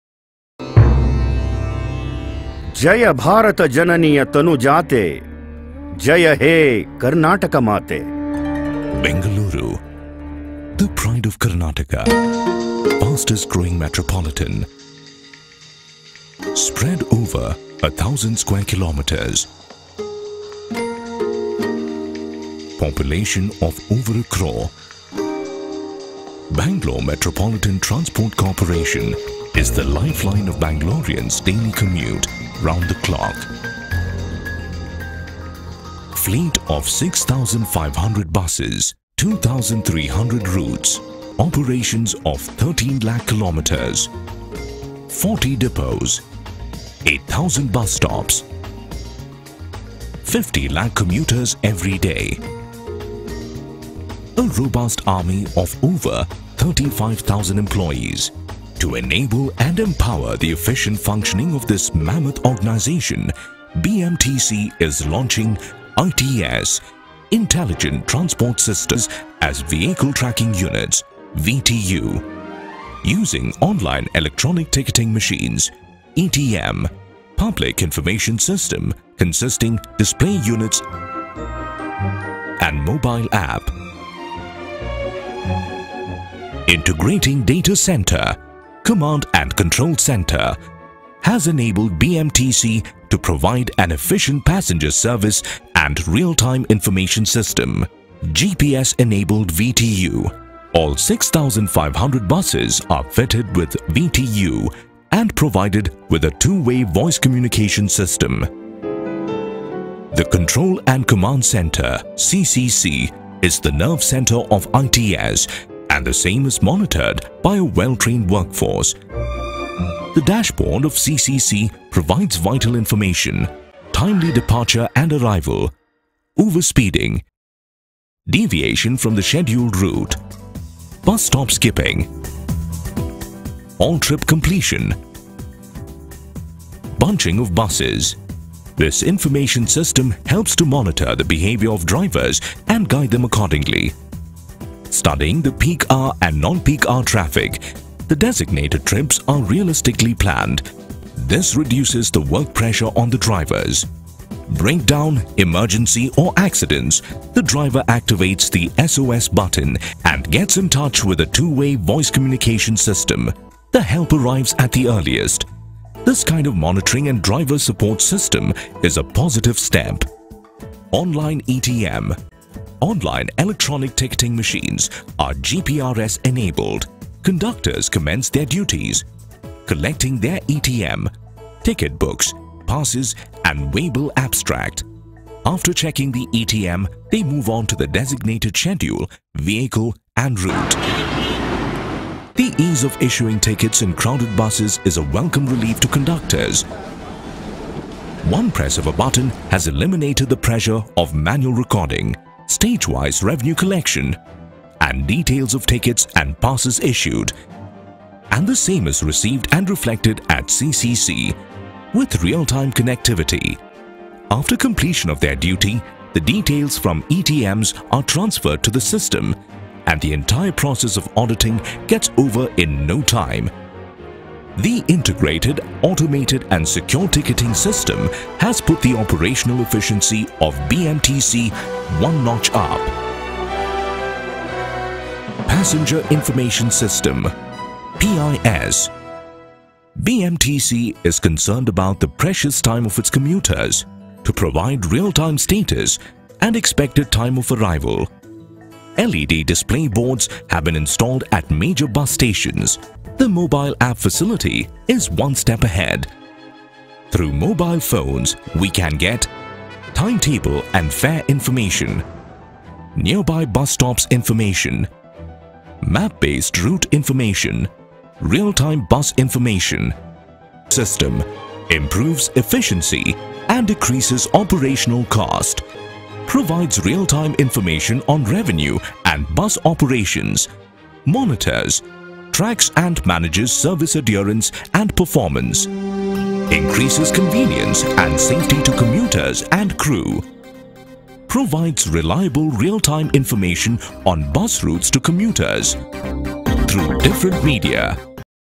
BMTC Intelligent Transport System
BMTC implemented Intelligent transport system for the city of Bangalore to disseminate real time information to commuters for their convenience